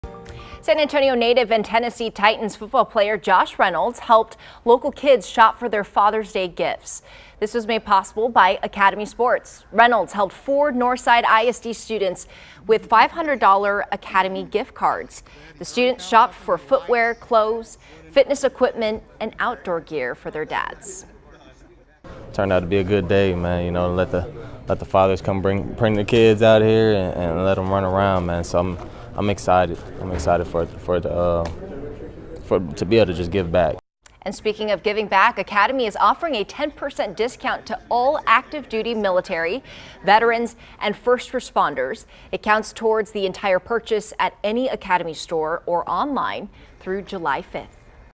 Titans WR Josh Reynolds hosts Father's Day shopping spree for San Antonio students
San Antonio native and Tennessee Titans wide receiver Josh Reynolds helped local kids shop for Father's Day gifts.